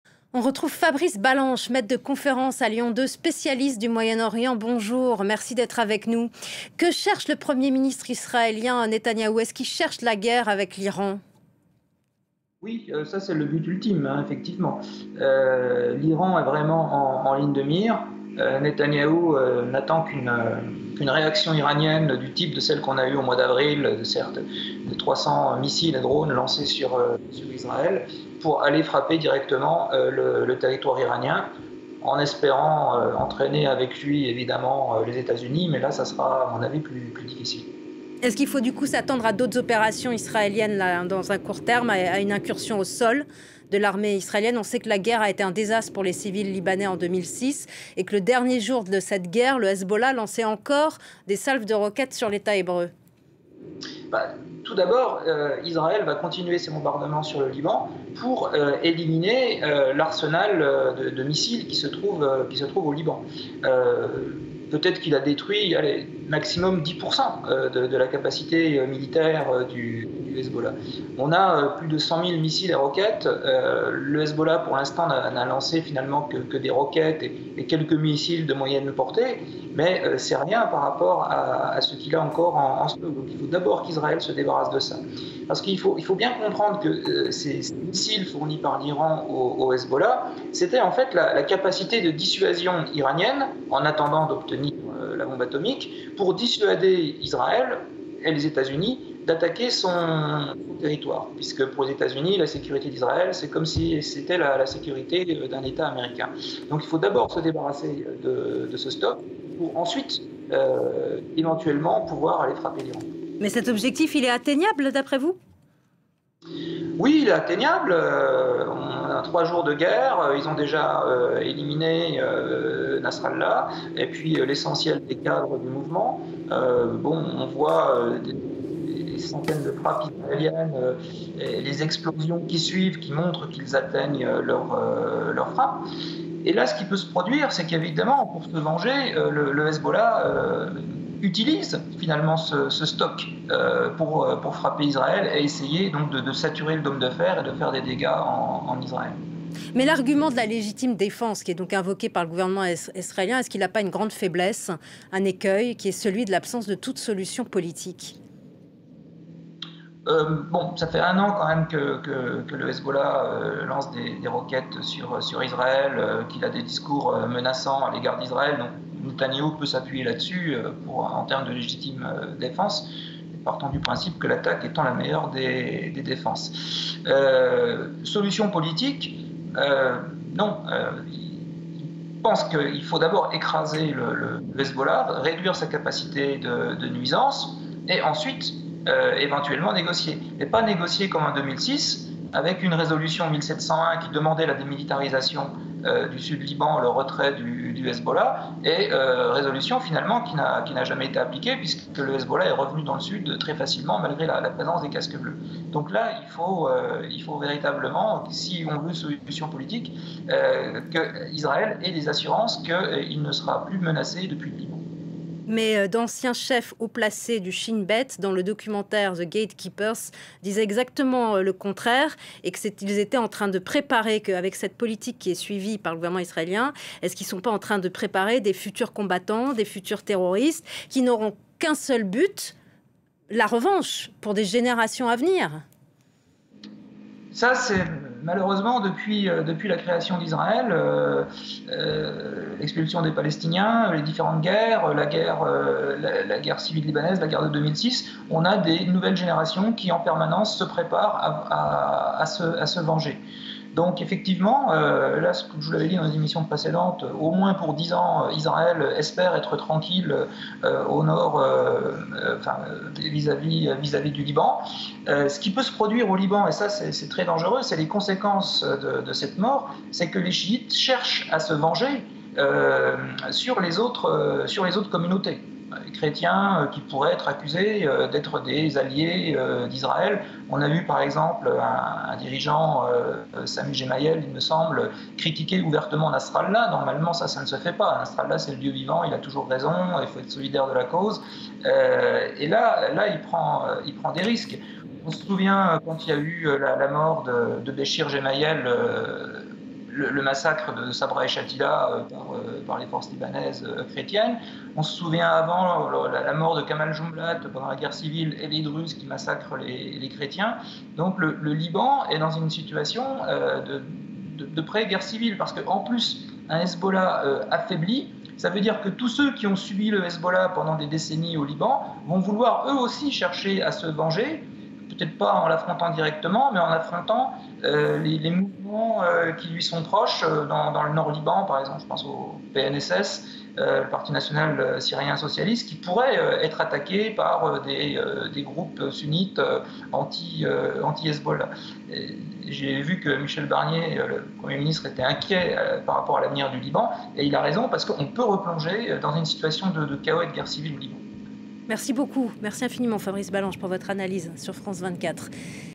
Le Premier ministre israélien Benjamin Netanyahu cherche-t-il la guerre avec l'Iran ? • FRANCE 24
L'armée israélienne a annoncé samedi avoir tué le chef du Hezbollah, Hassan Nasrallah, lors d'une frappe aérienne vendredi sur le quartier général du mouvement libanais dans la banlieue sud de Beyrouth. L'objectif d'Israël est-il de déclencher une guerre avec l'Iran ? Judith Grimaldi a posé la question à Fabrice Balanche, maître de conférence à l'Université Lyon 2.